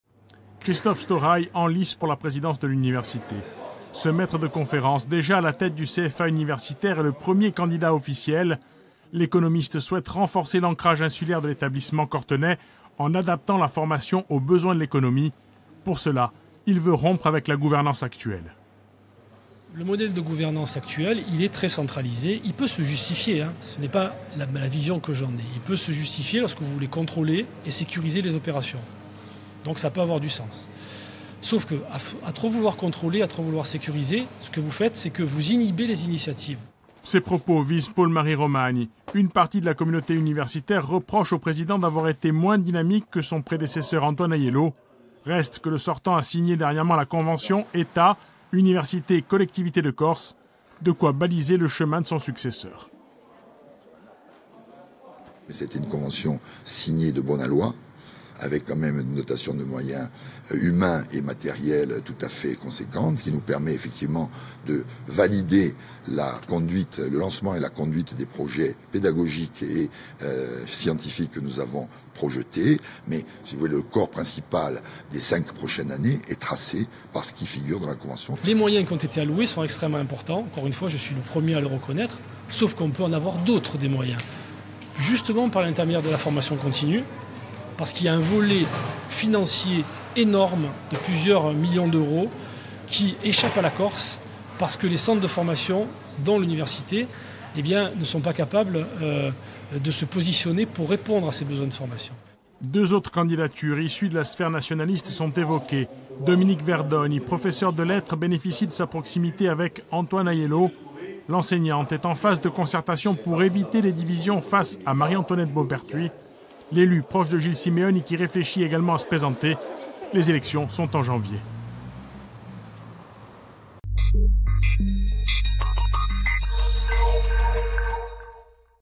Corte : qui sera le futur président de l'Université de Corse ?
A 4 mois des élections à la présidence de l'Université de Corse, plusieurs noms de candidats sont évoqués pour succéder à Paul-Marie Romani. Le premier à officialiser sa candidature est Christophe Storai. Ancien directeur de l'IUT et actuel responsable du centre de formation alternance en universitaire. Il présente son projet.

Intervenants -Christophe Storaï, candidat à la présidence de l'Université de Corse // Pierre-Marie Romani, Président de l'Université de Corse

Equipe - Dominique Moret // Franck Rombaldi